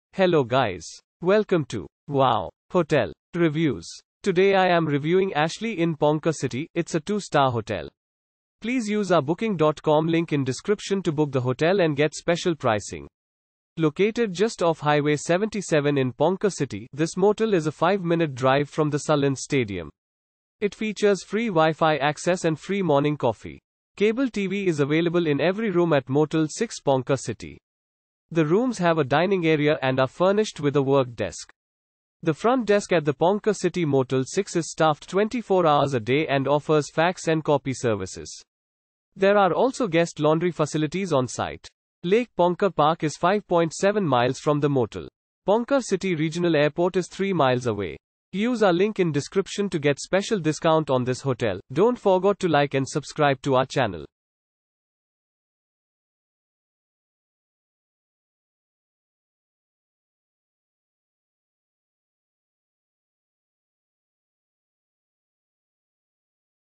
Ashley Inn Ponca City Review - Ponca City , United States of America
Ashley Inn Ponca City

Located just off Highway 77 in Ponca City, this motel is a 5-minute drive from the Sullins Stadium. It features free Wi-Fi access and free morning coffee.
Cable TV is available in every room at Motel 6 Ponca City. The rooms have a dining area and are furnished with a work desk.
The front desk at the Ponca City Motel 6 is staffed 24 hours a day and offers fax and copy services. There are also guest laundry facilities on site.
Lake Ponca Park is 5.7 miles from the motel. Ponca City Regional Airport is 3 miles away.